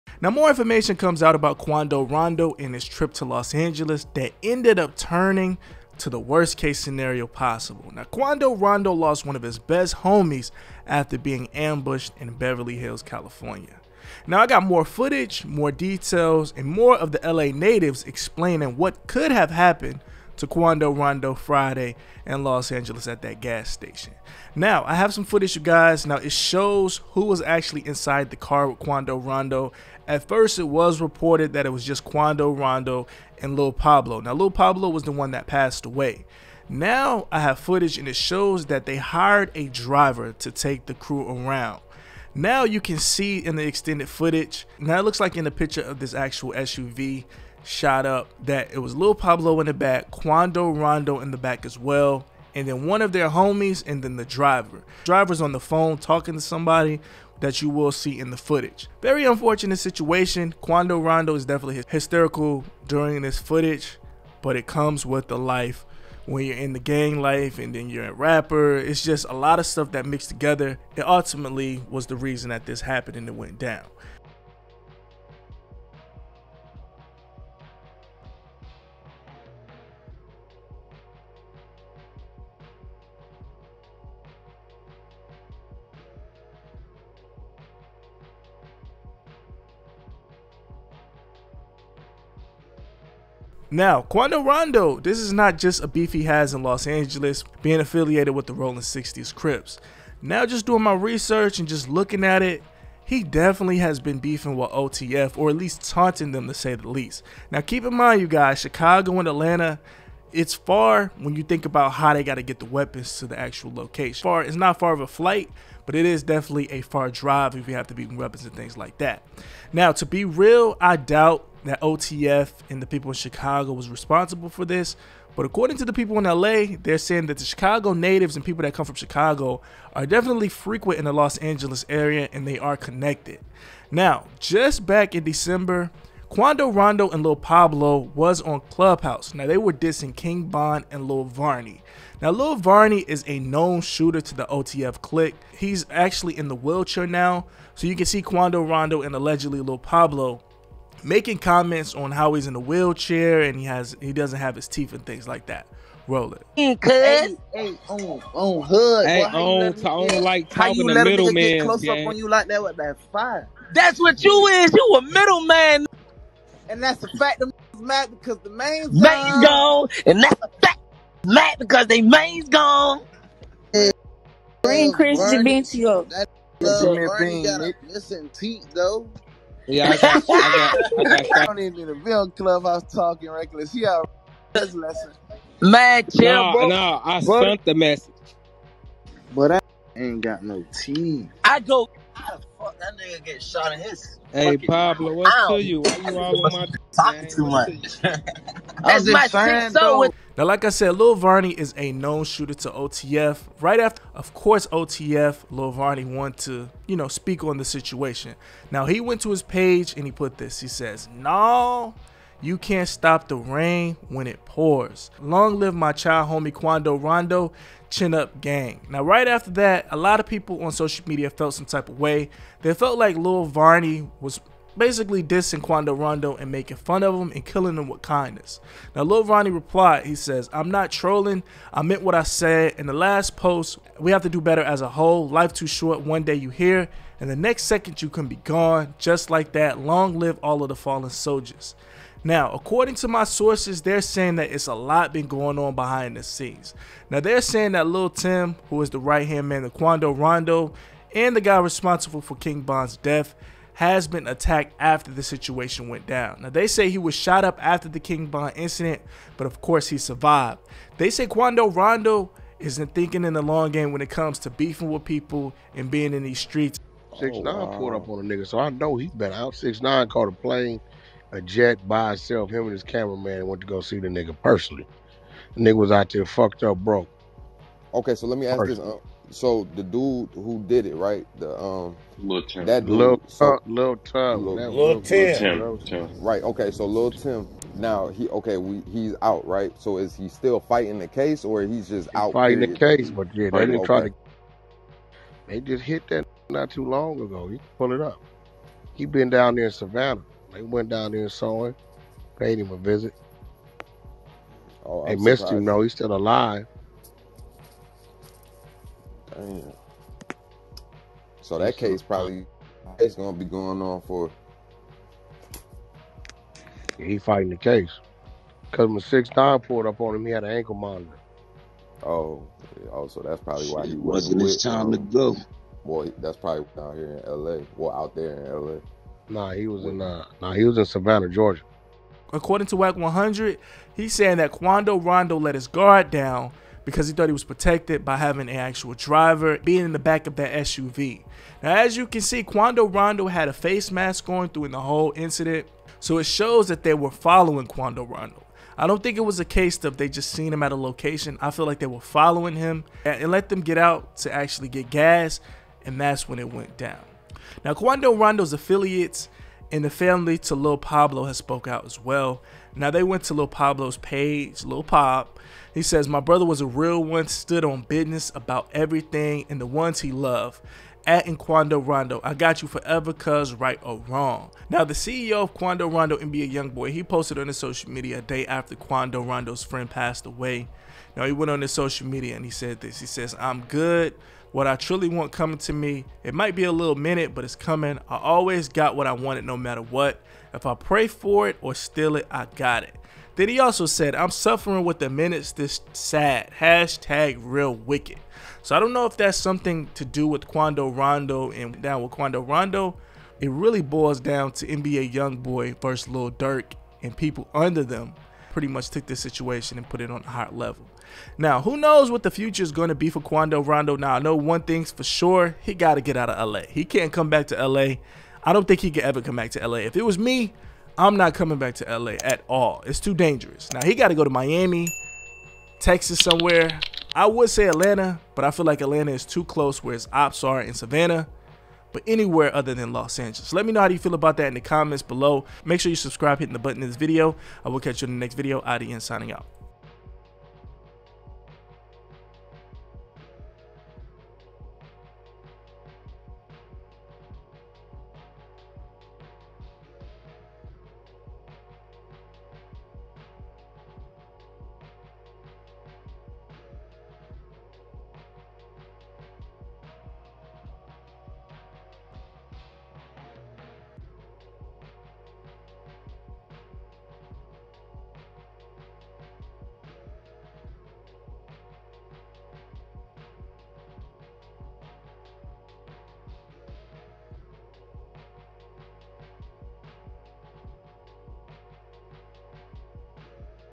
Unreleased Audio of Quando Rondo & Lul Pab Celebrating the Passing the King Von
1:14 More Angles of the Aftermath
2:26 Rondo & Lul Pab Dissing King Von After Passing
4:26 Von's Affiliates Been SLIDING for Von in Georgia
6:14 NBA Youngboy Reacts to Rondo's Incident